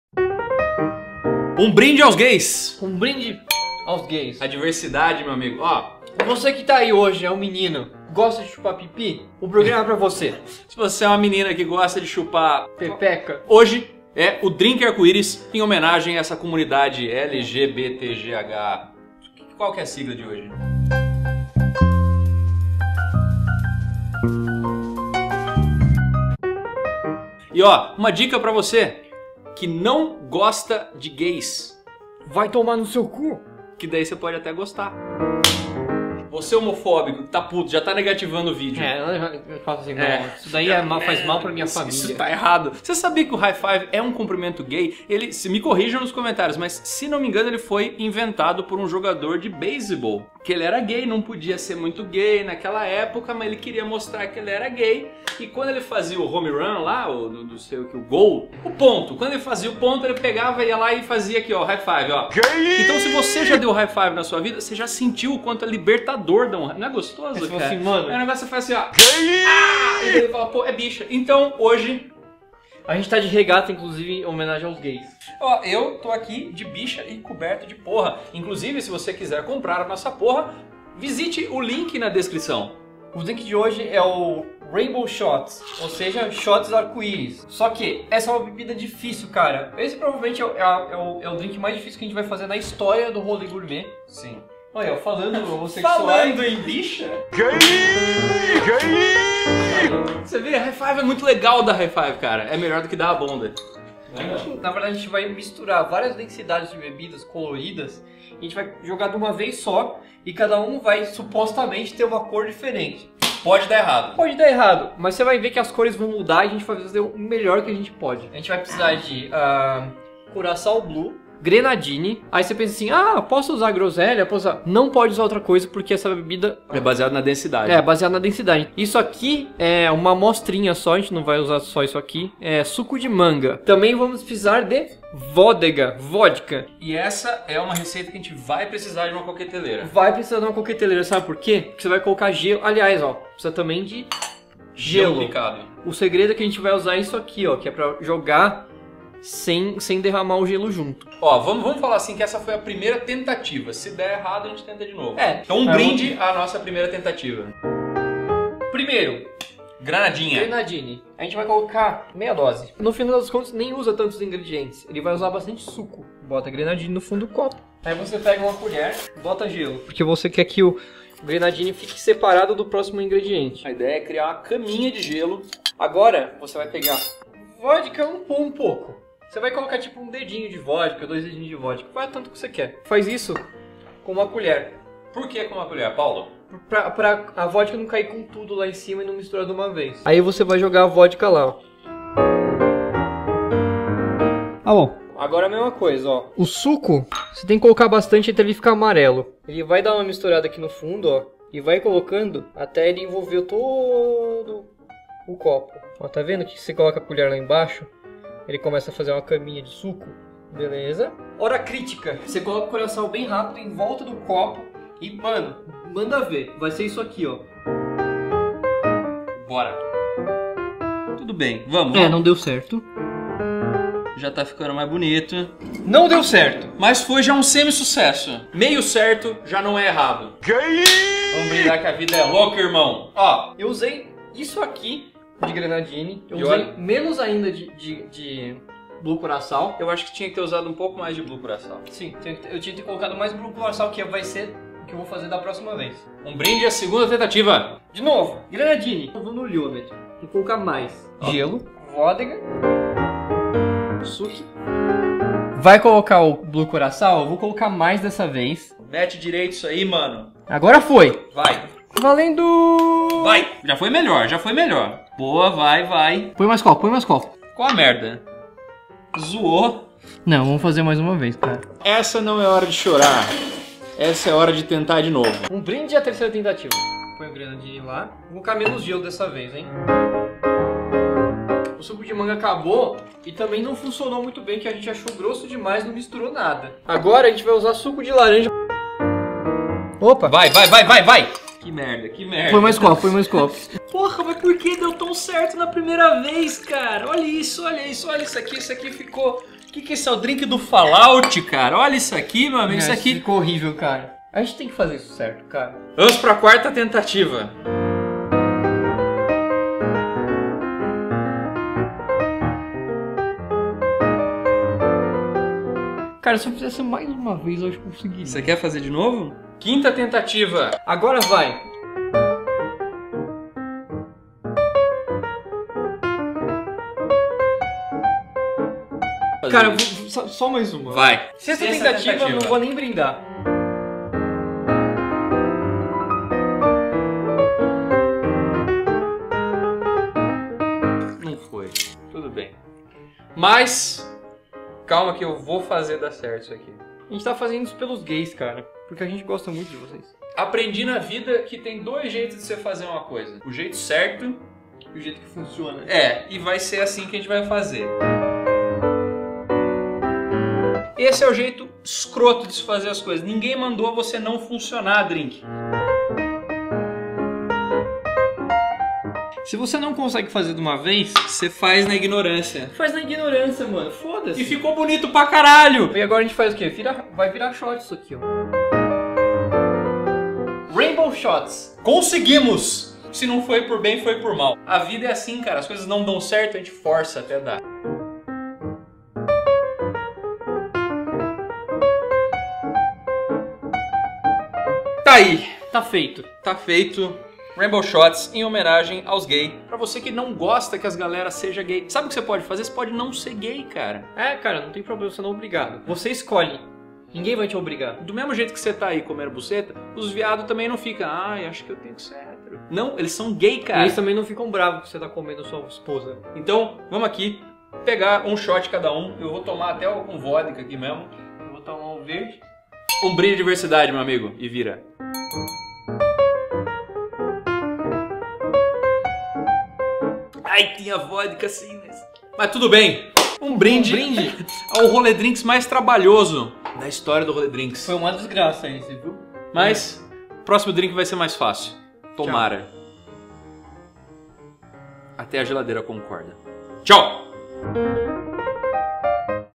Rainbow Shots, o Drink do Arco-Íris!
CURTA o vídeo!) Rainbow Shots, o drink mais difícil e COLORIDO do Rolê Drinks até hoje! Não posso dizer que foi um sucesso, mas foi nossa homenagem à diversidade sexual!

Compre Porras, Canecas e Chaveiros do Rolê Gourmet!

[ Rainbow Shots ]

Ingredientes:
- Grenadine
- Curaçao Blue
- Suco (de manga ou laranja)
- Vodka
- Gelo picado

Preparo:
- Em uma coqueteleira, ponha um xabláu de Grenadine, uma camada de gelo, um dedo de vodka, bastante suco, outro dedo de vodka e por último uma talagada de Curaçao Blue
- NA MESMA HORA sirva em copinhos de shot um ao lado do outro
- Se não der certo, vá na marra e misture as cores copo a copo porque FODA-SE, o que importa é que vai ficar bonito